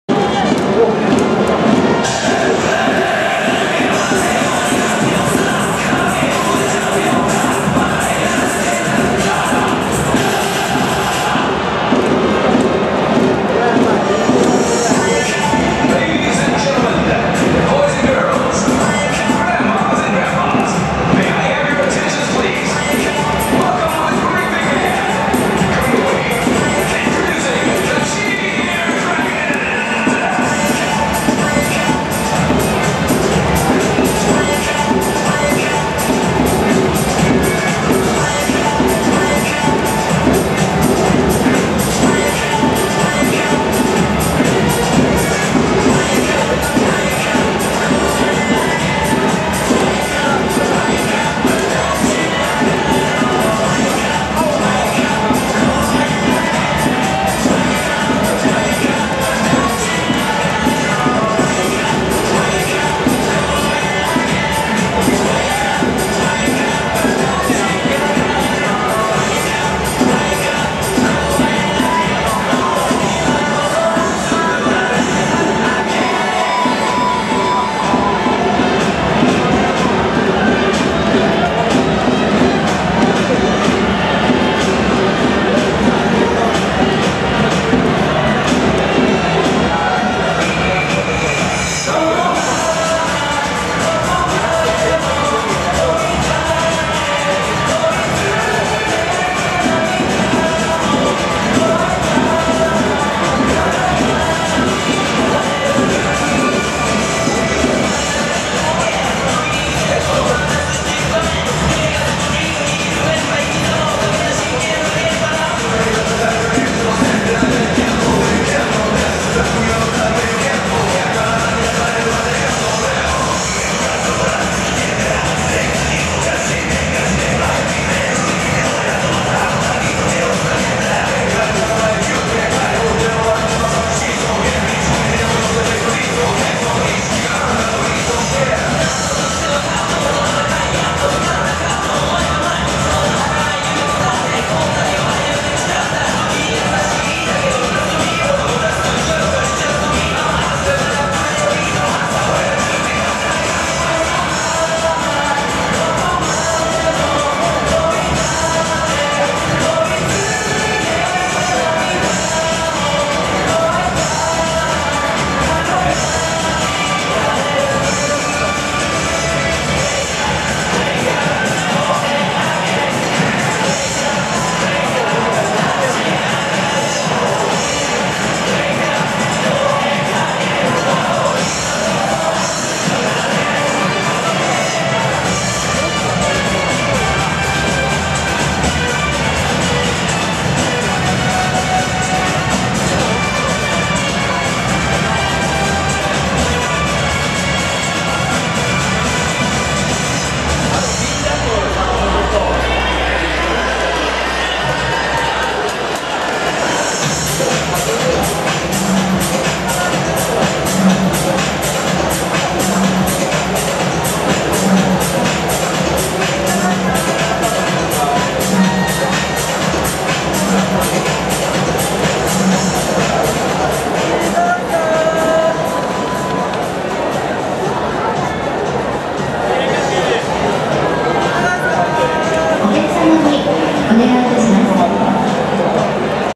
11/08/30 チアドラゴンズ2011 ＯＰダンス with トラッキー
チアドラゴンズ2011とシャオロン、パオロン、ドアラによるオープニングダンス

トラッキーも参加です